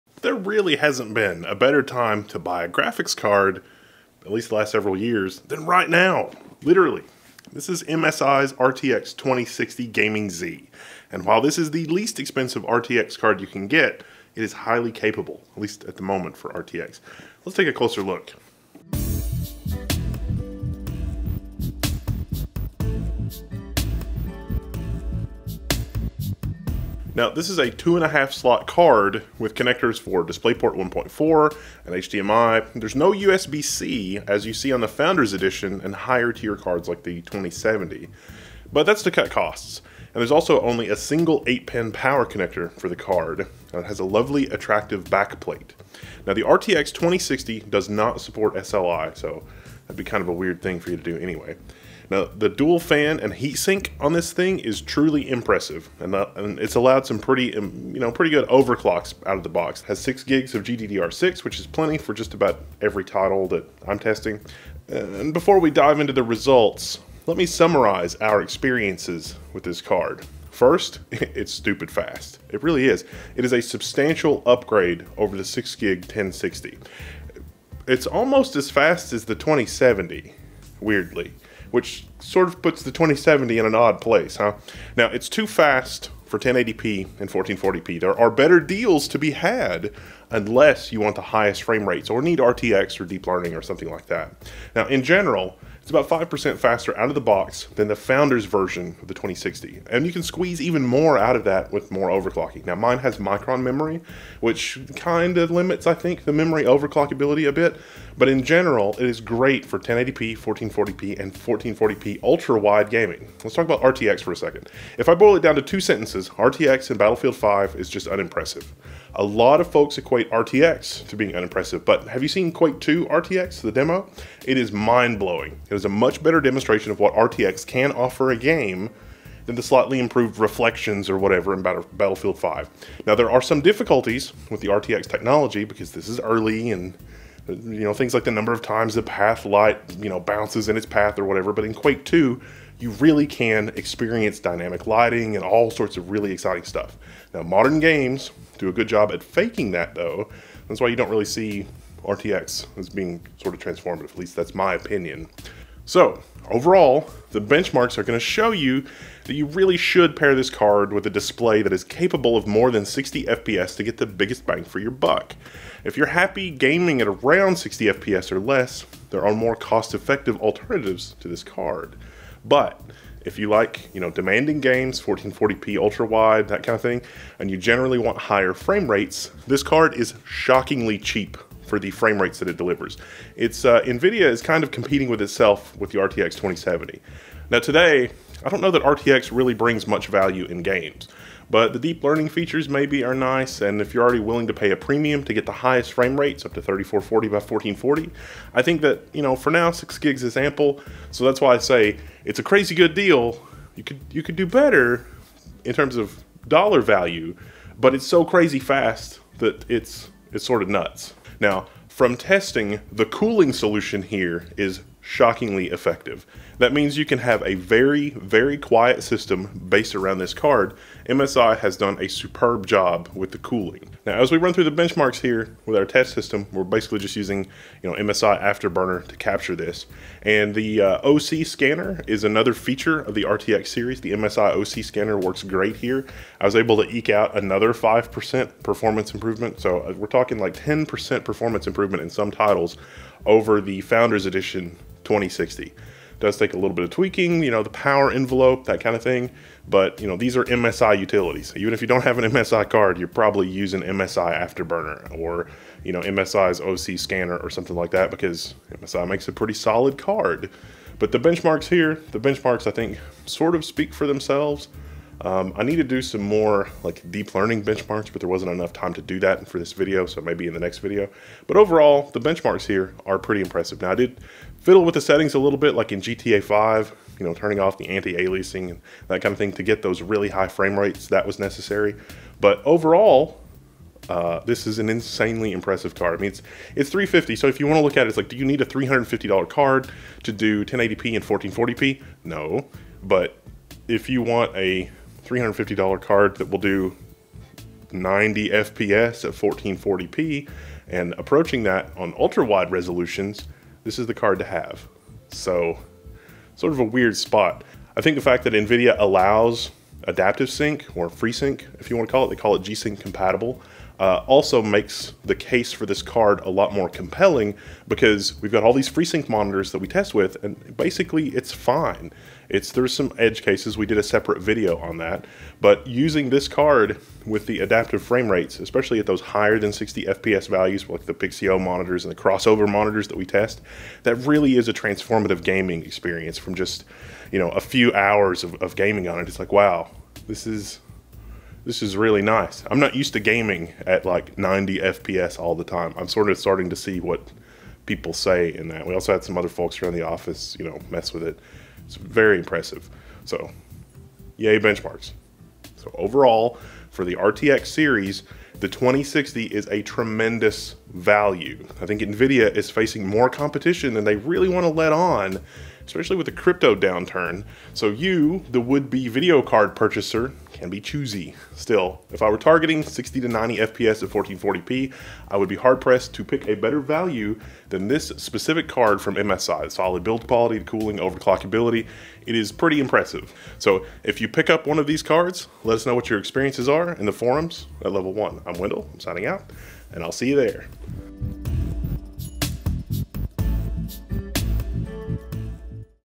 MSI Gaming Z RTX 2060 Review + Benchmarks!
RTX: it was fine in Quake2 but RTX is too slow to be usable in anything else. In case that wasn't clear. otherwise the high frame rates are nice when paired with a high refresh g-sync compatible monitor.